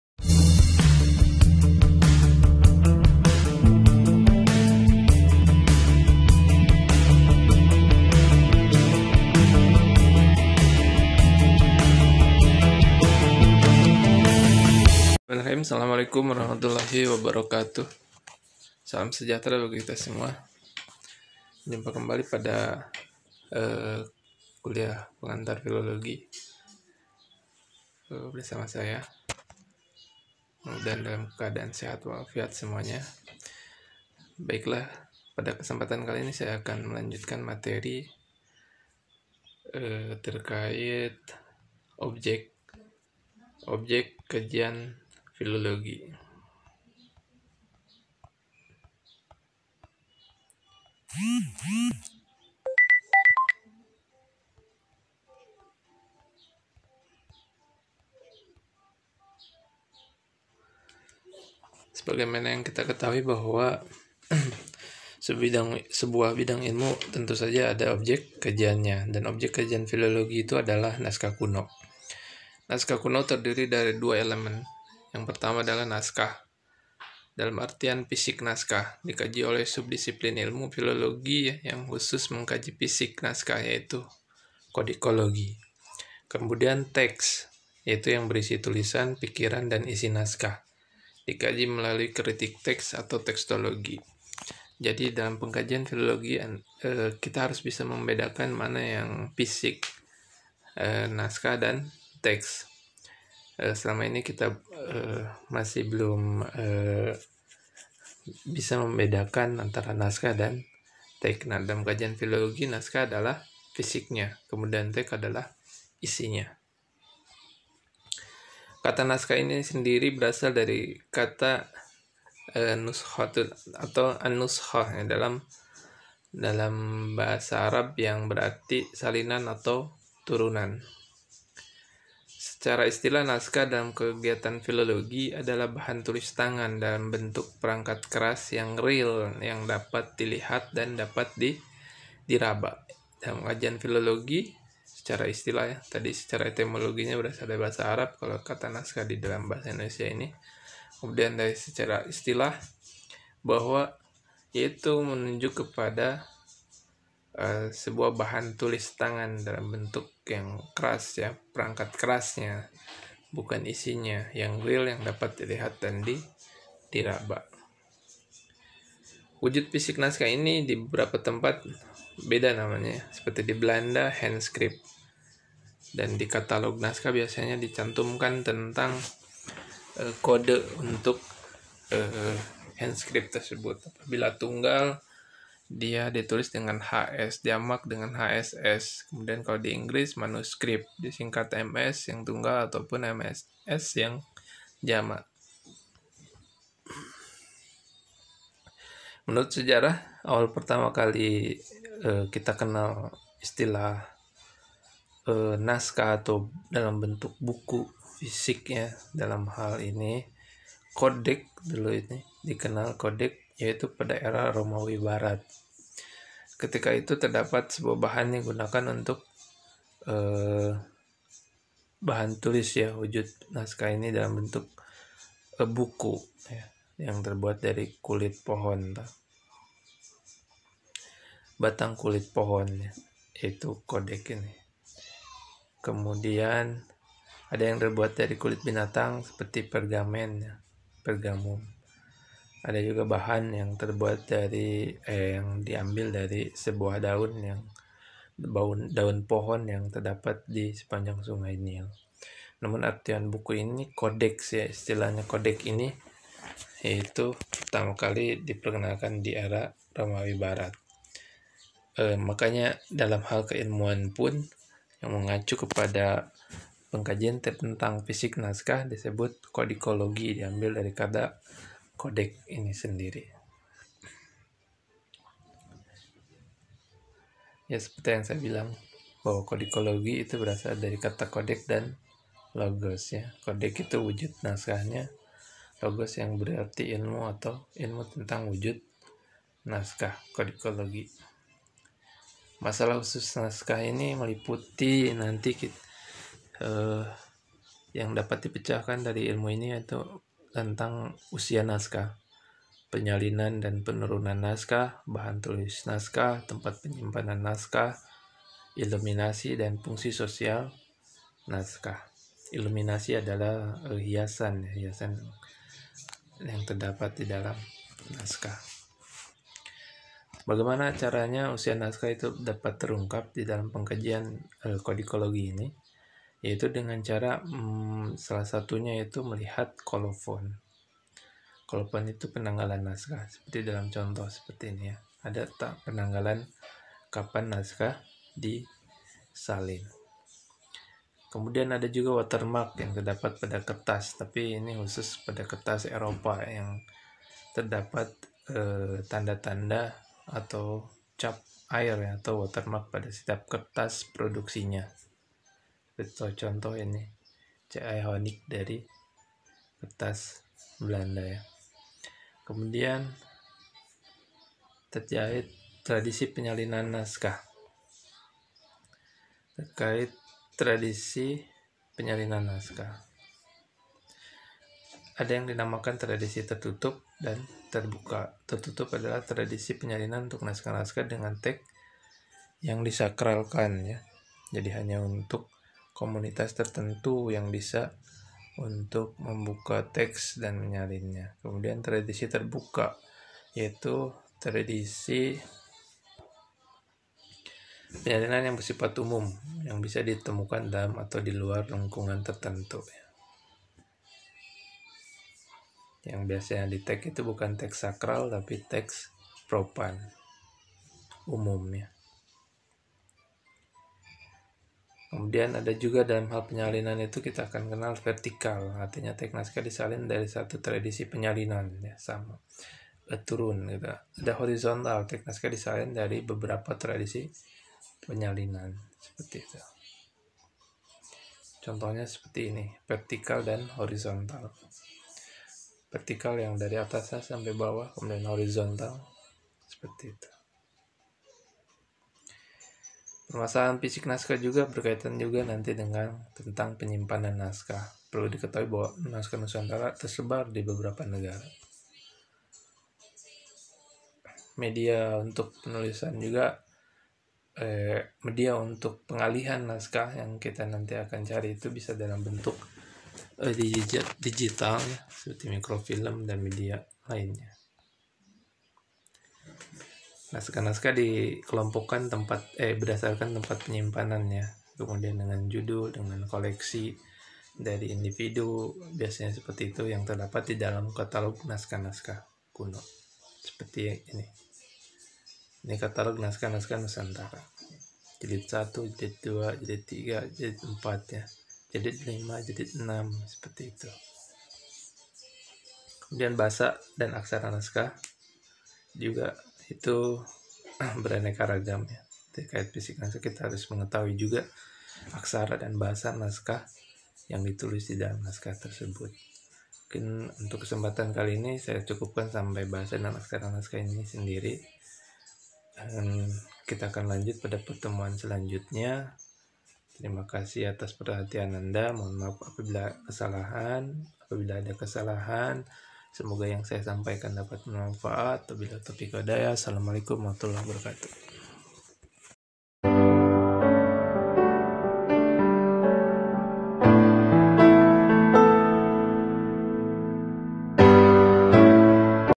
Kuliah Filologi bag.2
Video ini berisi materi  kuliah filologi pertemuan ke-2.